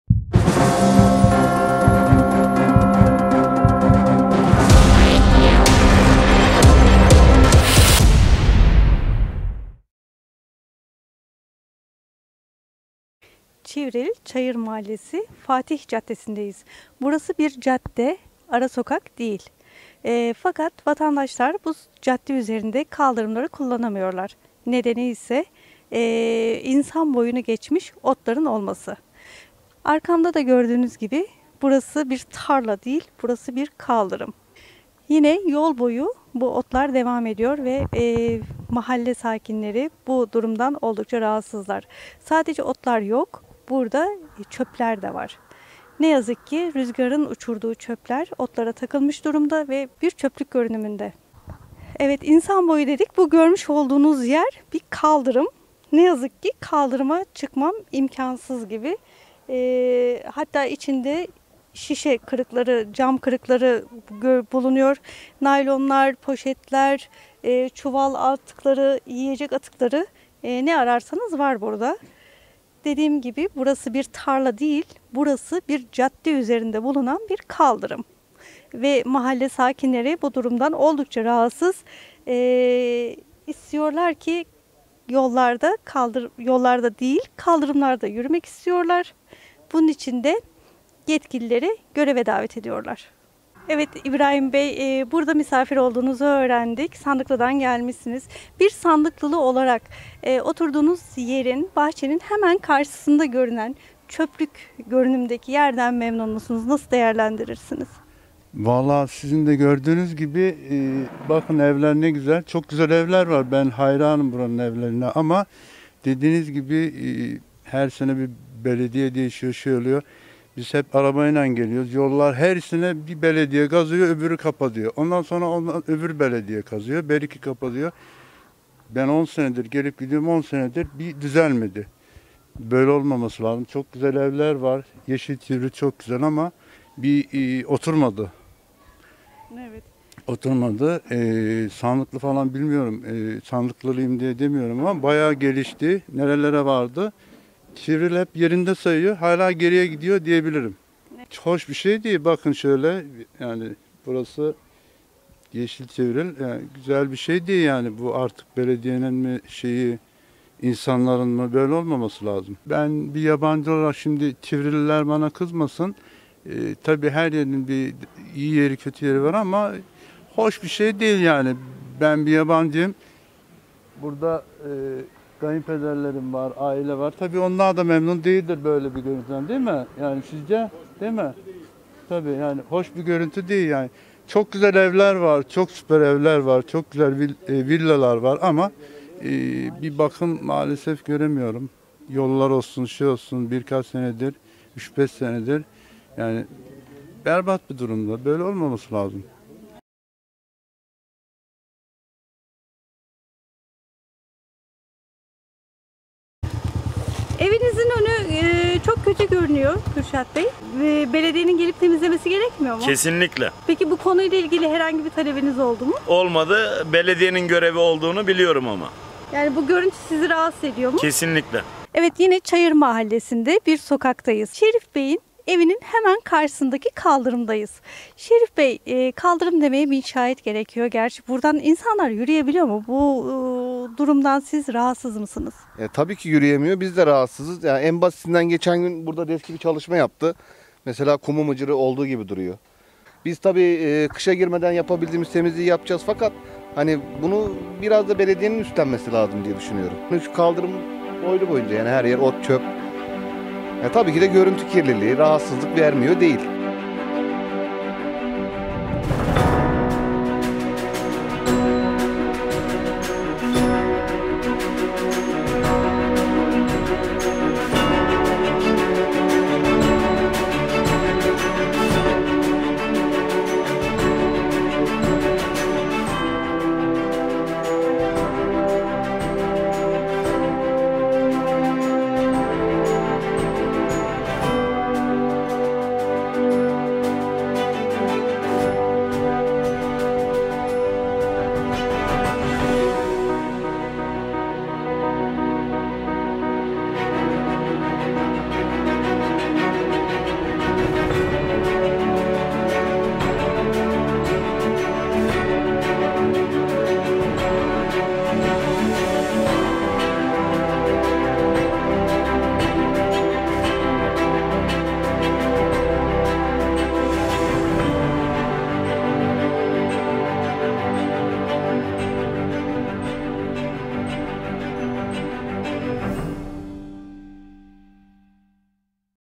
Çayır Mahallesi Kaldırımları
Çivril Çayır Mahallesi kaldırımlarının içler acısı durumunu mahalleli değerlendiriyor